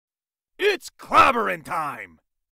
Marvel Snap Audition: The Thing
This was my audition that was shortlisted for The Thing for Marvel Snap. The audition was just his one, signature line. I was pretty happy with how this one turned out.